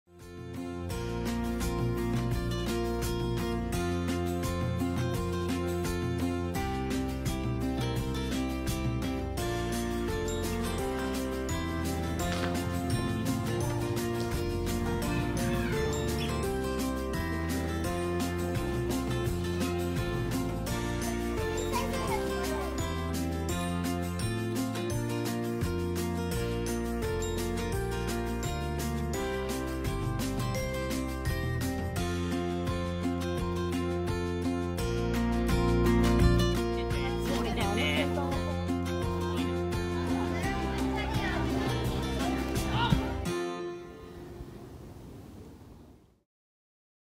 【平和の象徴】元気いっぱいの子供達の声が響く筑波山
秋真っ盛りお天気も良く筑波山は元気な子供達でいっぱいでした。
元気いっぱいの子供達は平和である象徴。
無心に一生懸命に筑波山登山をしている子供達から元気を貰いました。
鳩は平和の象徴ですが
何故鳩が平和の象徴なのか
よくわかりません
世界を見れば大人の戦争紛争で子供達は泣いています
元気いっぱいの子供達を見ていると今ここは平和である事を実感しました。